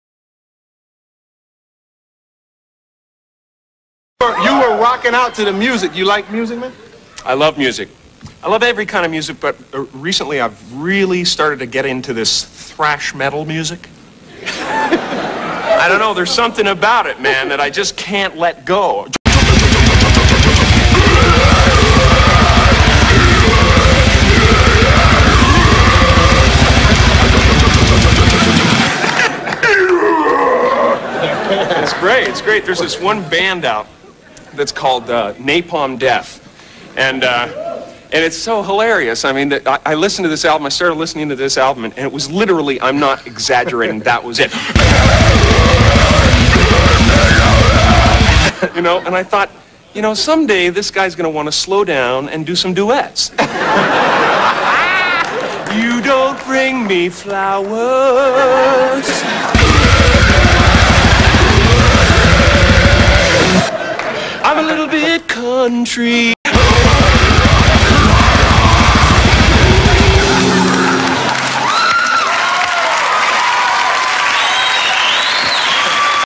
Jim "Fucking Metal" Carrey - SINGS NAPALM DEATH
Fala galera, fiz uma brincadeira com a lendária entrevista de Jim Carrey ele cita que aprecia o trabalho da banda Napalm Death

James Eugene "Jim" Carrey é um ator, comediante, roteirista, produtor e pintor canadense, conhecido por interpretar papéis cômicos em filmes como Ace Ventura, Debi e Loide, O Máscara, O Pentelho, O Mentiroso, Eu, Eu Mesmo e Irene, Todo Poderoso, Desventuras em Série e As Loucuras de Dick e Jane.

Napalm Death é uma banda de metal extremo da Inglaterra, formada no ano de 1981 por Nicholas Bullen e Miles Ratledge no vilarejo de Meriden, próximo a cidade de Birmingham.

Equipamento usado na gravação: guitarra Kramer com captador Dimarzio X2N, interface de audio Audiobox Presonus, Logic Pro, Toontrack Ezmix e Toontrack Ezdrummer.

Hey guys, I made a joke with the legendary Jim Carrey interview he says he appreciates the work of the band Napalm Death

James Eugene "Jim" Carrey is a Canadian actor, comedian, screenwriter, producer and painter, known for playing comic roles in films like Ace Ventura, Debi and Loide, The Mask, The Twilight, The Liar, Me, Myself and Irene, Almighty, Series Unfortunate Events and Dick and Jane's Madness.

Napalm Death is an extreme metal band from England, formed in 1981 by Nicholas Bullen and Miles Ratledge in the village of Meriden, near the city of Birmingham.

Equipment used in recording: Kramer guitar with Dimarzio X2N pickup, Audiobox Presonus audio interface, Logic Pro, Toontrack Ezmix and Toontrack Ezdrummer.